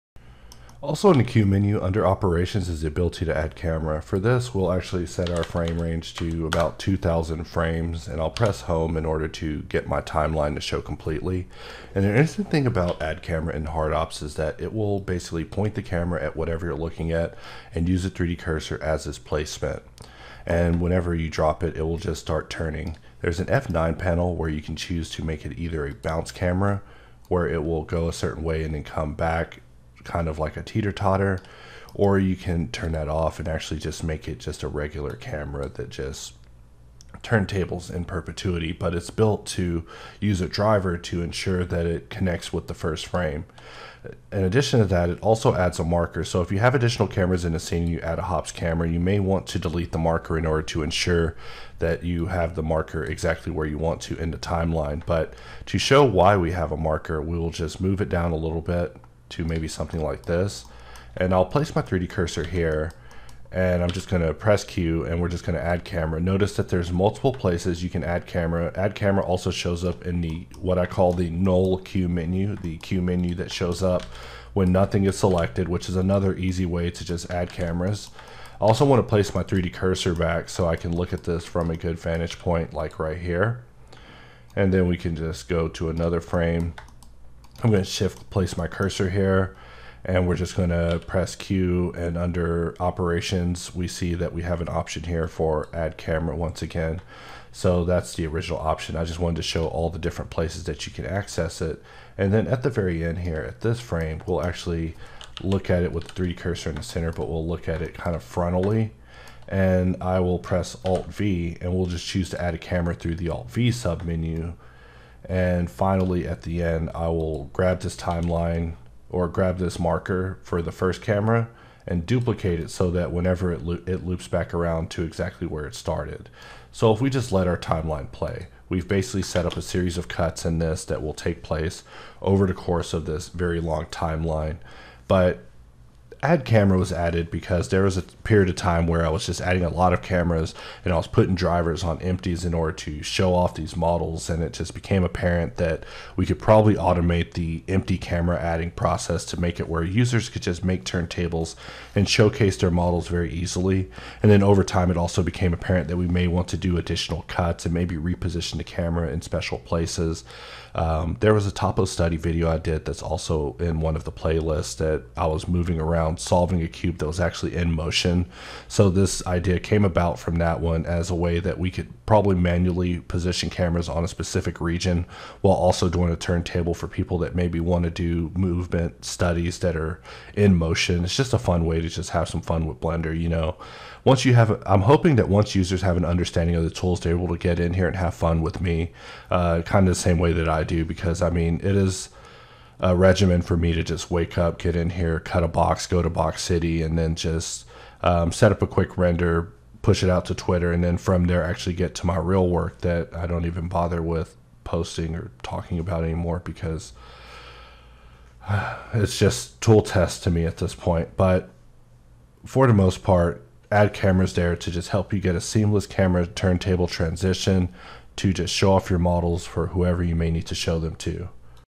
hardops 986X - Add Camera (40)
Add camera is the ideal way to make a quick turntable on the fly. With the bounce cam an alternative way to present is also available. I also aim to show the 3d cursor support and how to get the most out of our quick camera.

Try... anywhere. Just try.